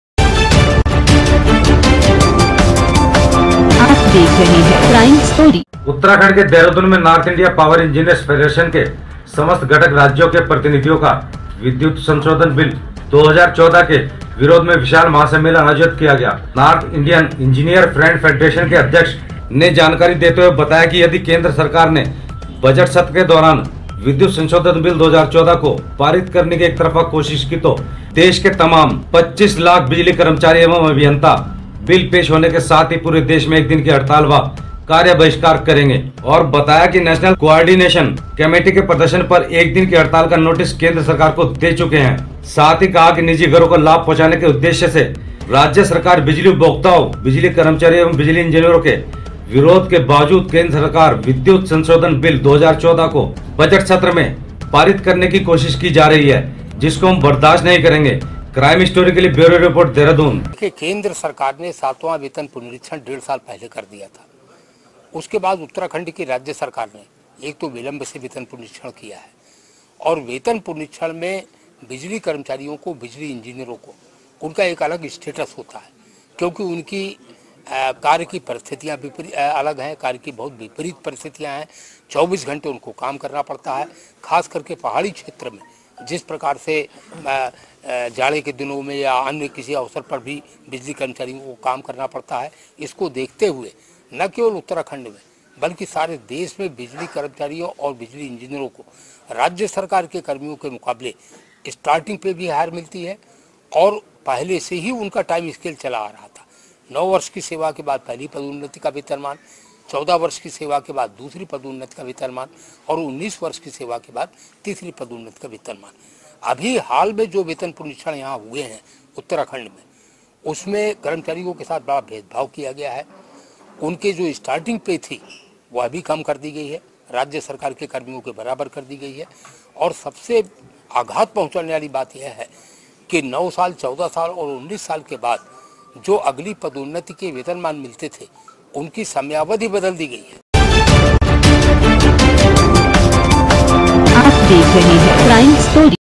नॉर्थ इंडिया पावर इंजीनियर्स फेडरेशन ने किया महासम्मेलन आयोजित
नॉर्थ इंडिया पावर इंजीनियर्स फेडरेशन के समस्त घटक राज्यों के प्रतिनिधियों का विद्युत संशोधन बिल 2014 के विरोध में विशाल महासम्मेलन आयोजित किया गया। नॉर्थ इंडियन इंजीनियर फ्रेंड फेडरेशन के अध्यक्ष ने जानकारी देते हुए बताया कि यदि केंद्र सरकार ने बजट सत्र के दौरान विद्युत संशोधन बिल 2014 को पारित करने की एकतरफा कोशिश की तो विरोध स्वरूप देश के तमाम 25 लाख बिजली कर्मचारी एवं अभियंता बिल पेश होने के साथ ही पूरे देश में 1 दिन हड़ताल व कार्य बहिष्कार करेंगे।और बताया कि नेशनल कोआर्डिनेशन कमेटी के प्रदर्शन अब 1 दिन की हड़ताल का नोटिस केंद्र सरकार को दे चुके है

साथ ही कहां की निजी घर निजी घरों को लाभ पहुंचाने के उद्देश्य से राज्य सरकार बिजली उपभोक्ताओं ,बिजली कर्मचारियों एवं बिजली इंजीनियरों के विरोध के बावजूद केंद्र सरकार विद्युत संशोधन बिल 2014 को बजट सत्र में पारित कराने की कोशिश की जा रही है।जिसको हम बर्दाश नही करेगे।